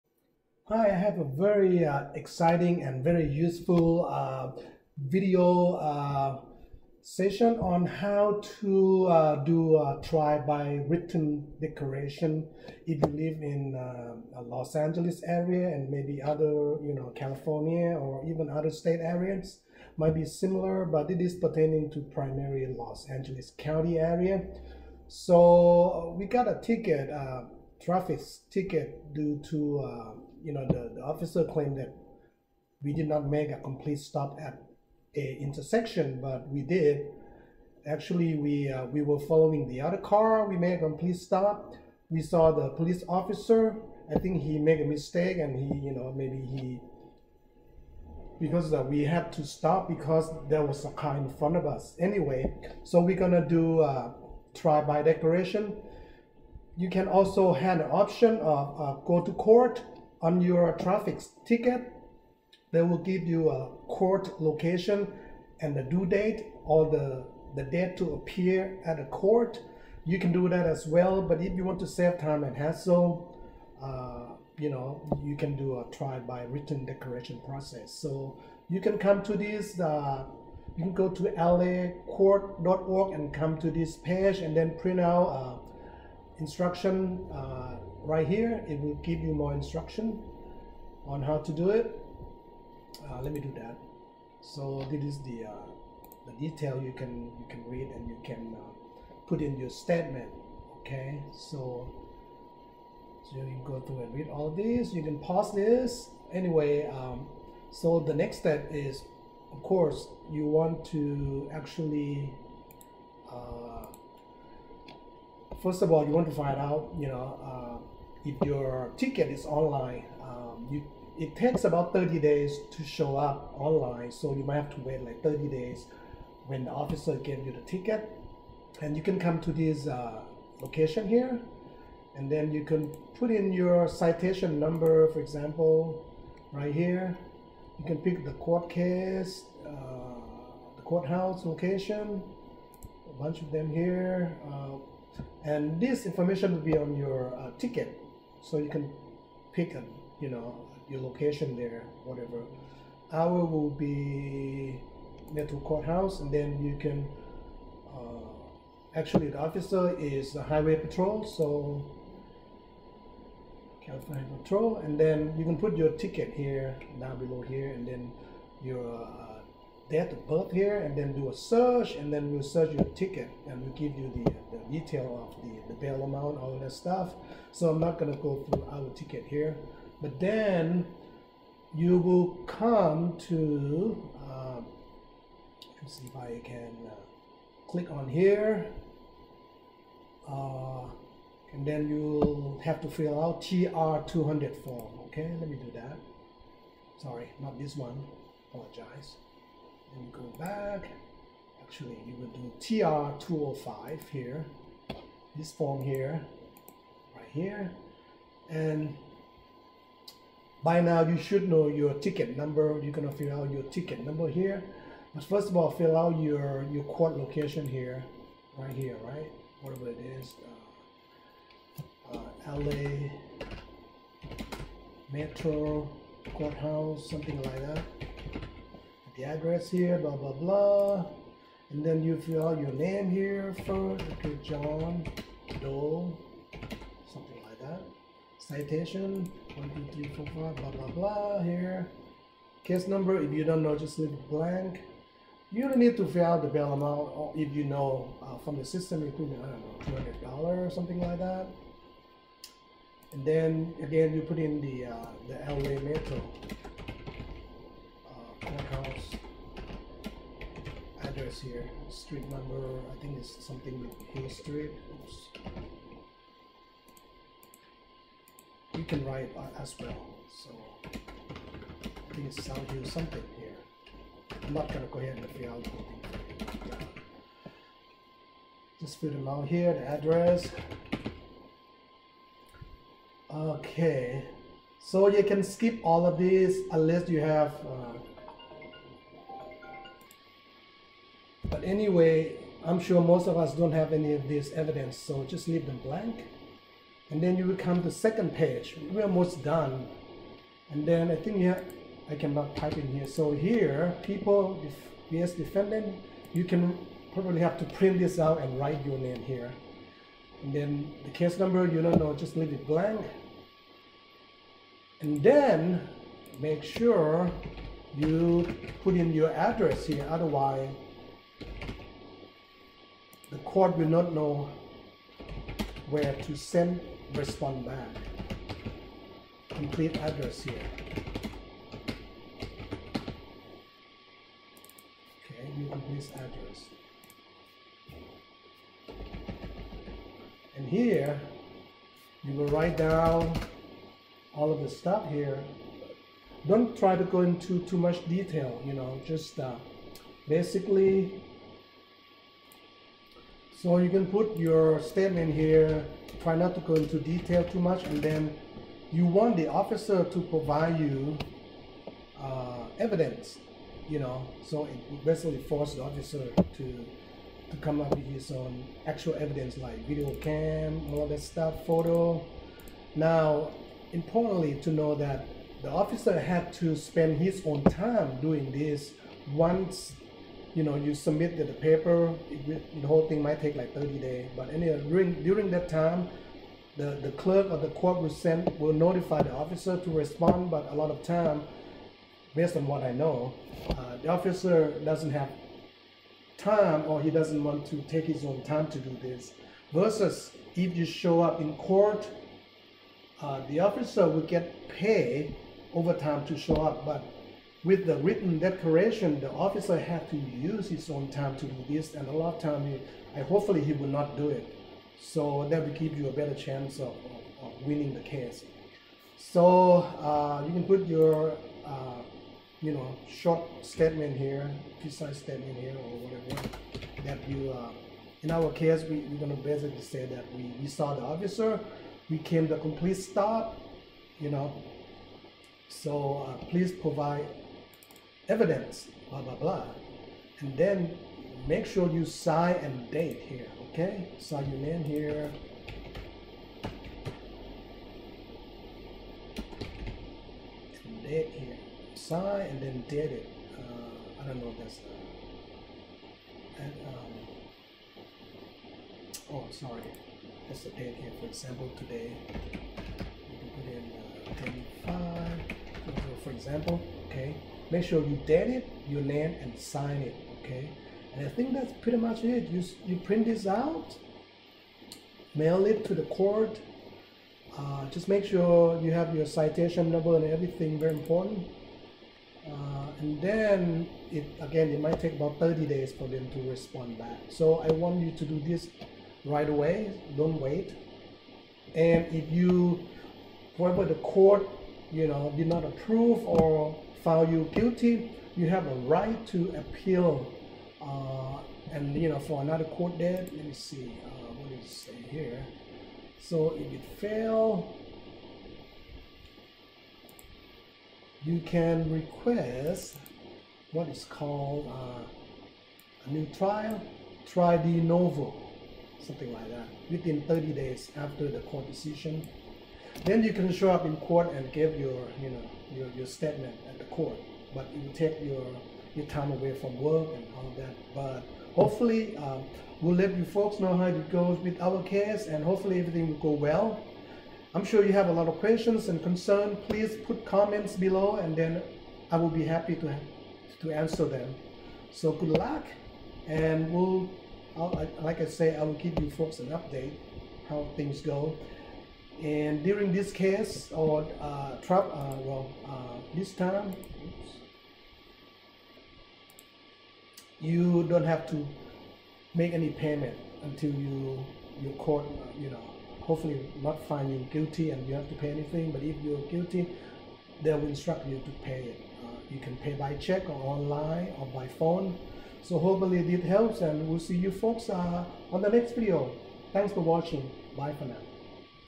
Los Angeles Traffic Ticket Contest - DIY Save Time & Win using Trial by Written Declaration
Please share your experience whether success or fail so we all can learn and improve the whole process.